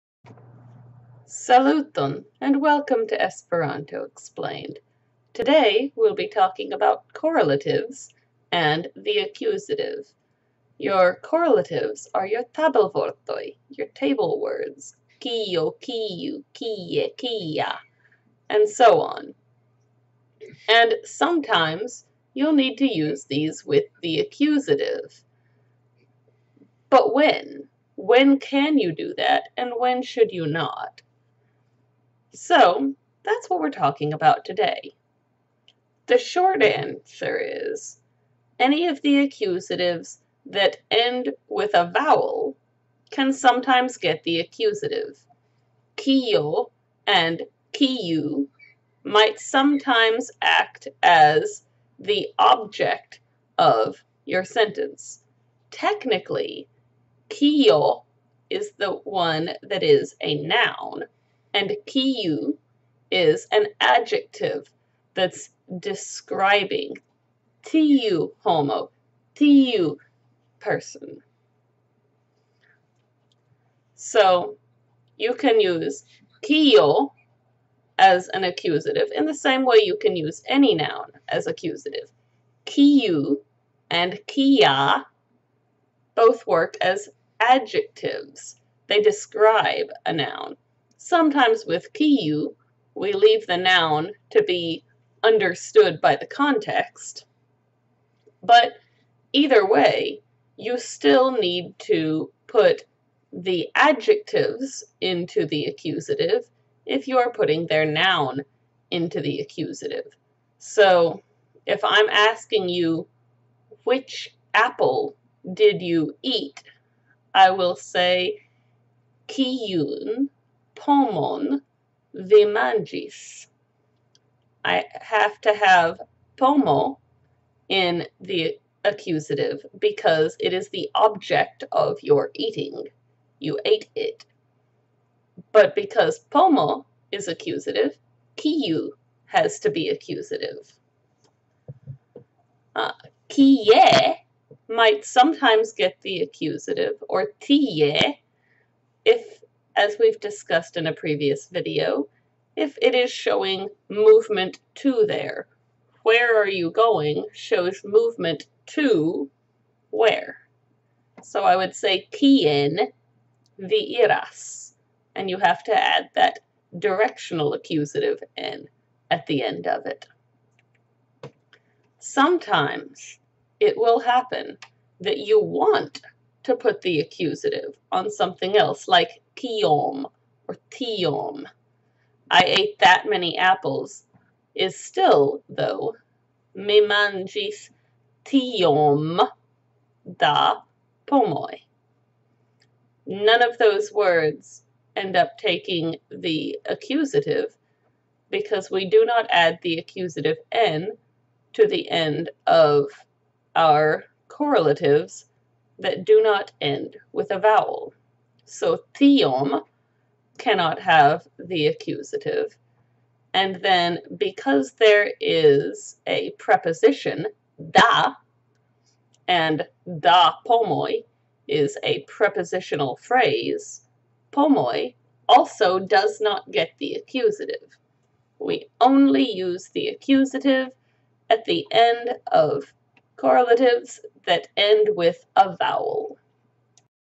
Esperanto Explained: Correlatives and Accusative
This video explains how the Esperanto correlatives work with the accusative -n.